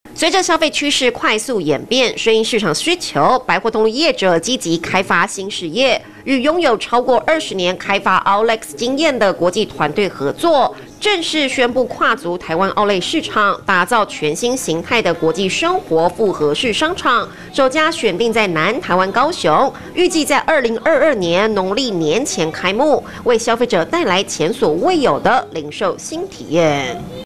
擴展新事業版圖 跨足台灣Outlet市場
隨著消費趨勢快速演變，順應市場需求，百貨通路業者積極發展新事業，與擁有超過20年開發outlets經驗的國際團隊合作，正式宣布跨足台灣outlet市場，打造全新型態的國際生活複合式商場，首家選訂在南台灣高雄，預計在2022年農曆年前開幕，為消費者帶來前所未有的零售新體驗。